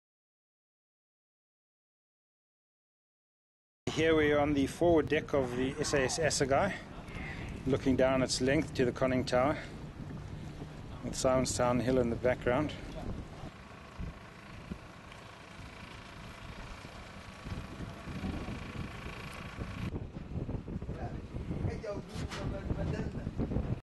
SAS Assegaai 3 - External.mp4
Brief external view of the SAS Assegaai Museum Submarine in the Simon's Town Naval Base in South Africa